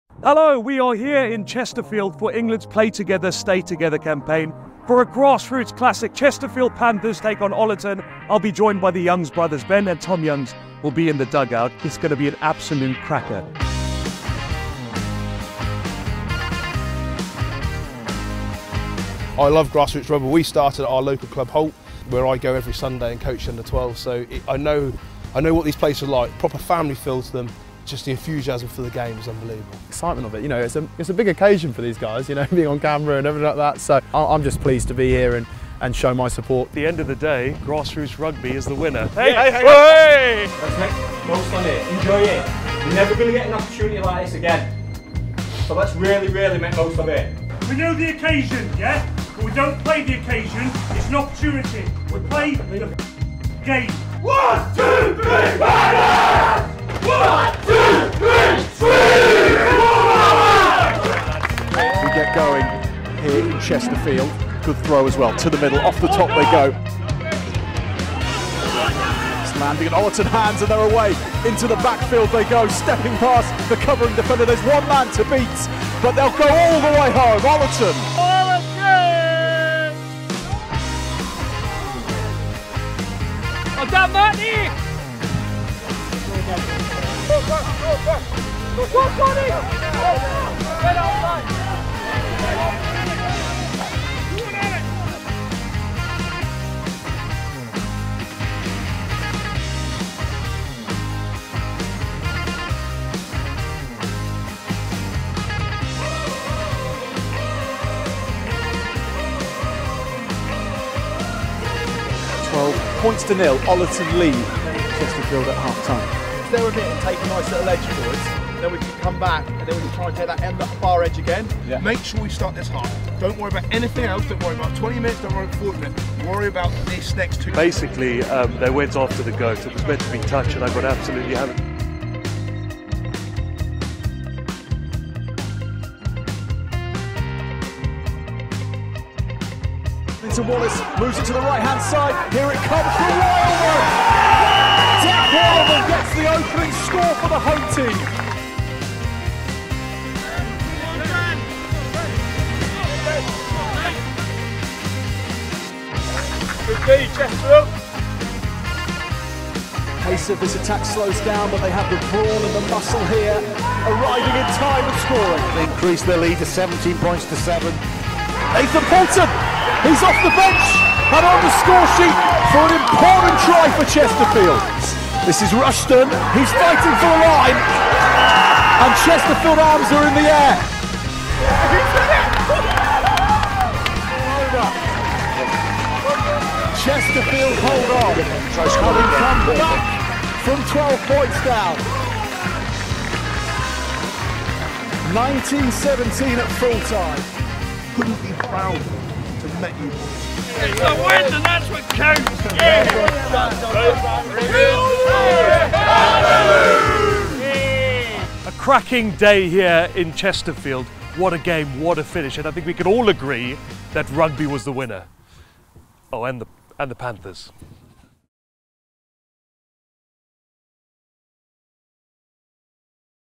Ben Youngs vs Tom Youngs: A grassroots rivalry 👀 Play Together Stay Together
Brothers, Ben and Tom Youngs team up with Chesterfield Panthers and Ollerton RUFC in a down to the wire, grassroots classic as part of our Play Together Stay Together campaign.